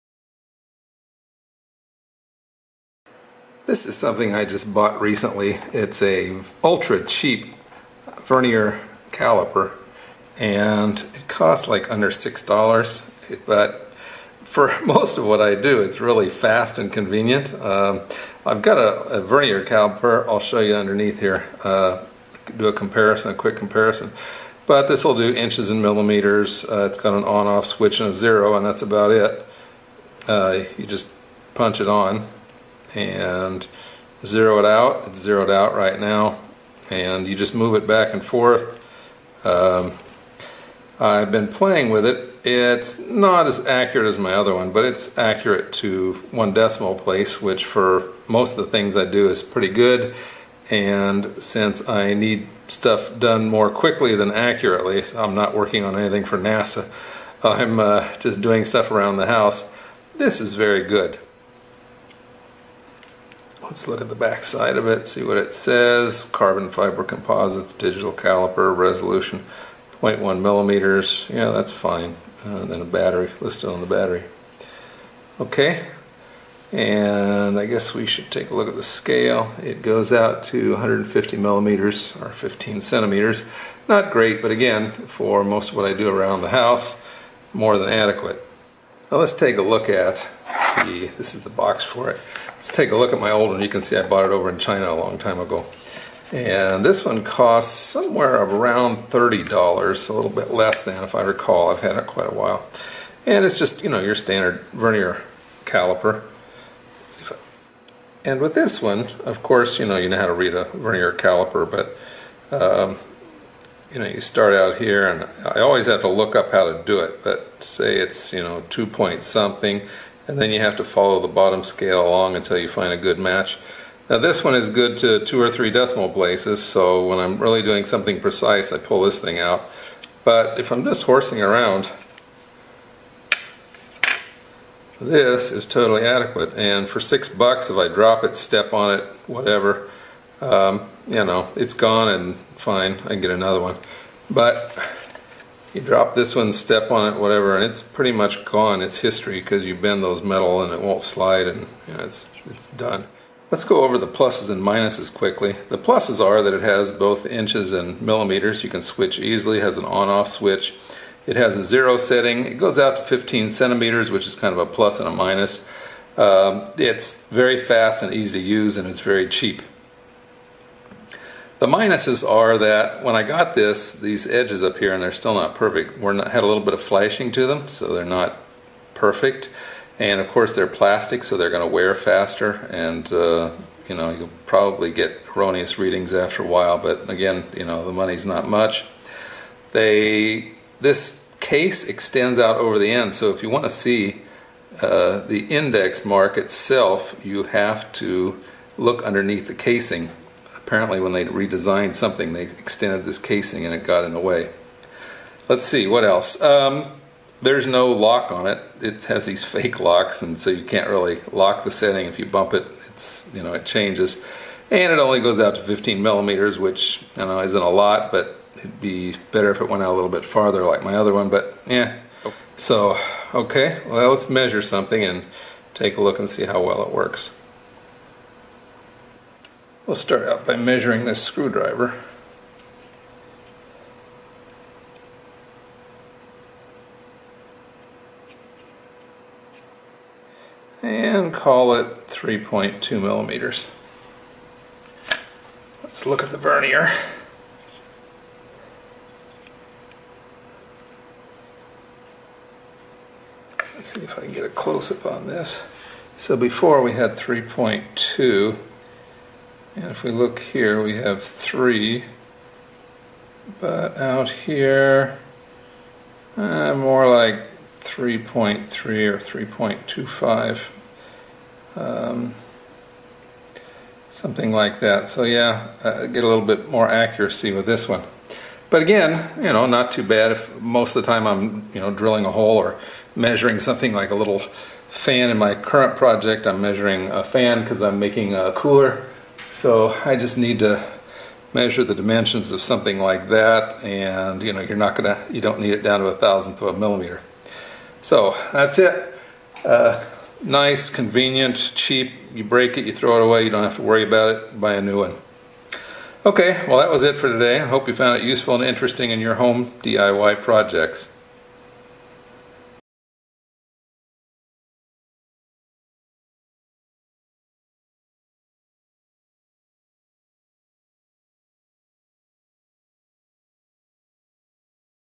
Vernier Calipers - Cheap vs Ultra-Cheap Digital
Comparing 2 calipers, cheap analog verses ultra-cheap digital.